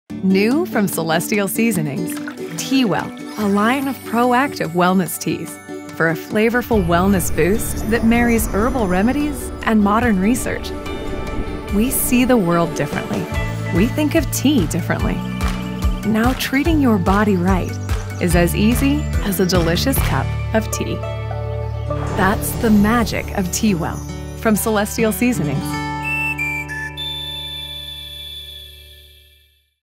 The Magic of TeaWell by Celestial Seasonings
Introducing TeaWell – a new line of proactive wellness teas from Celestial Seasonings. Now, treating your body right is as easy as a delicious cup of tea. That’s the Magic of TeaWell by Celestial Seasonings.